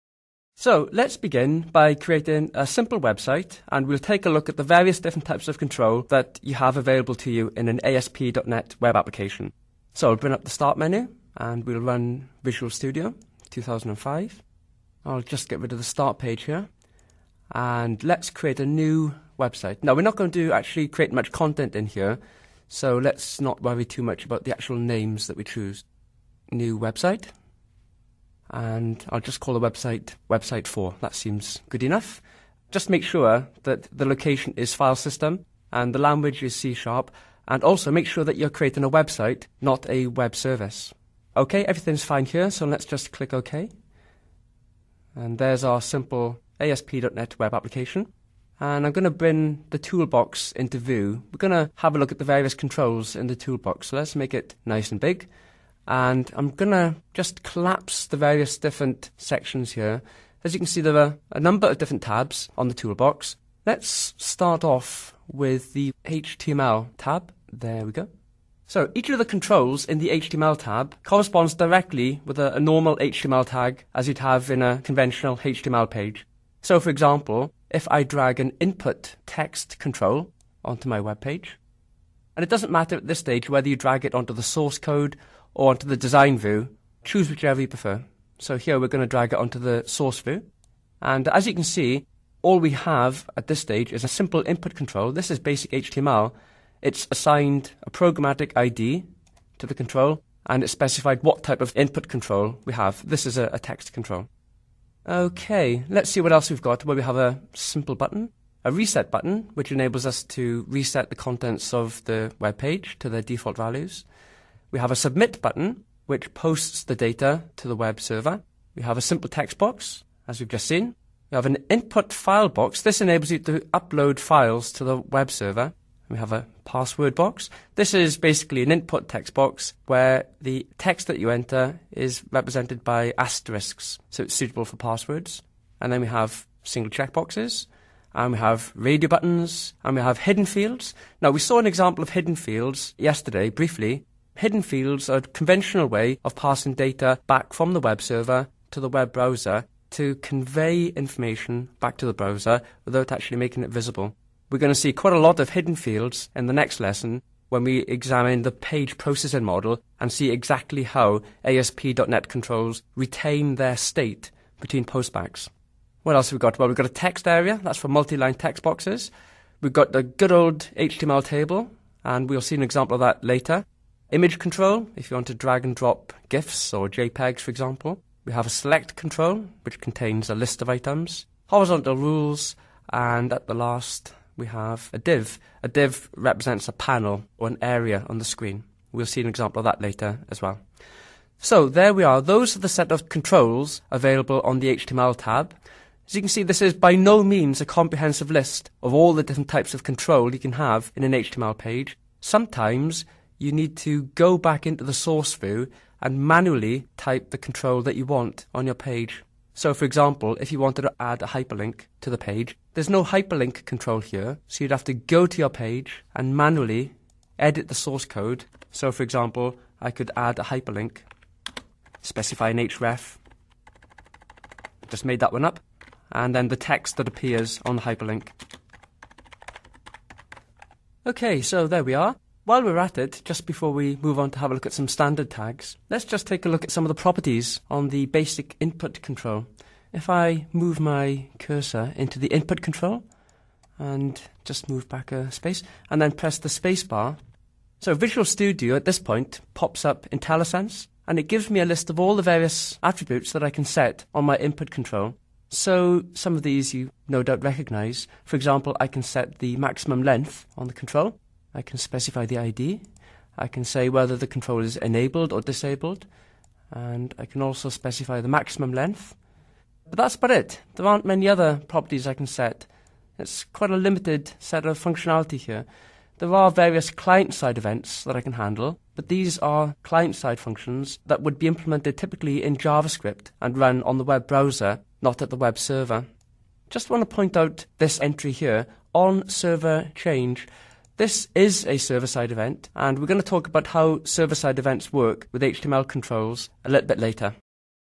How to Create a Web Site with Visual Studio HTML Control Tab @
Learn how to create a simple Web Site with various different types of controls in a ASP.NET Web Application. @